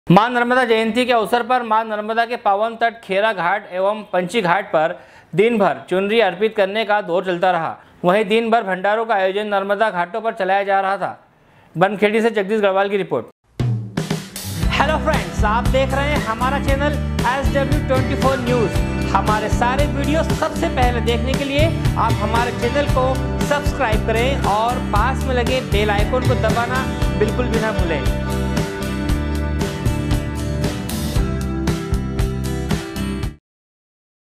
SW 24 News Madhyapradesh
मां नर्मदा जयंती के अवसर पर मां नर्मदा के पावन तट खैरा घाट एवं पंछी घाट पर दिनभर चुनरी अर्पित करने का दौर चलता रहा वहीं दिनभर भंडारे का आयोजन नर्मदा घाटों पर चलता रहा, बनखेड़ी से जगदीश गढ़वाल की रिपोर्ट